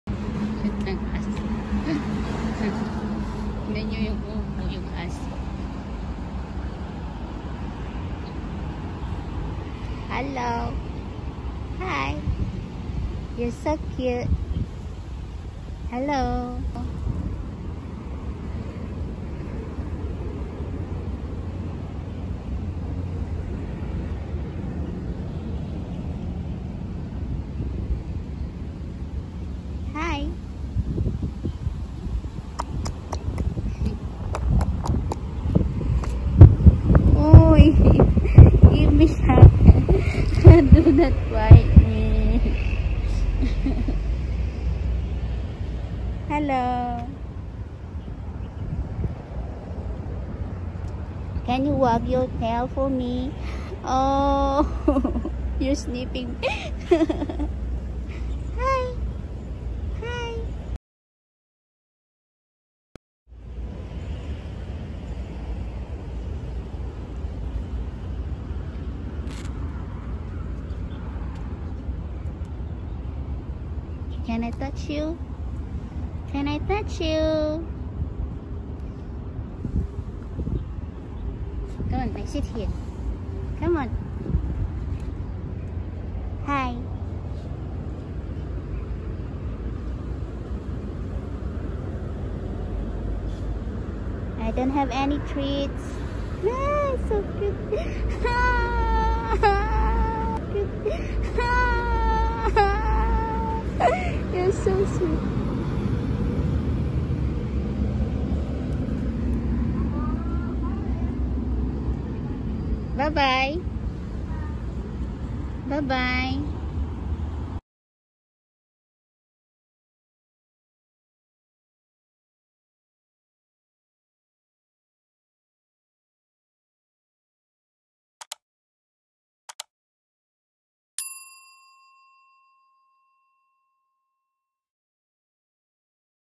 How to approach a dog for the first time? - sweet encounter of dog and human/shiba Inu breed
When I saw a dog that lies on its belly.

Howtoapproachadogthatyoudon'tknow?